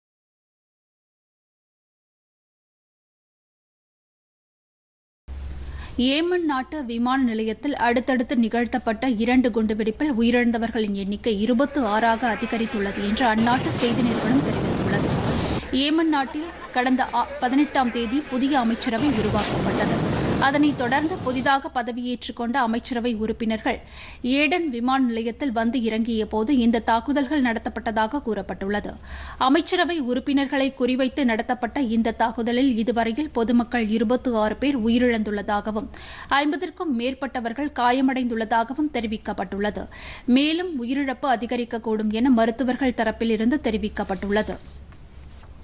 குண்டுவெடிப்பு - உயிரிழப்பு 26-ஆக உயர்வுPodhigaiTamilNews
ஏடன் விமான நிலைய குண்டுவெடிப்பில் உயிரிழந்தோர் எண்ணிக்கை 26-ஆக உயர்வு - ஏமனில் புதிதாக பதவியேற்ற பிரதமர் மற்றும் அமைச்சர்கள் உயிர் தப்பினர்.Visit : TamilNews - Doordarshan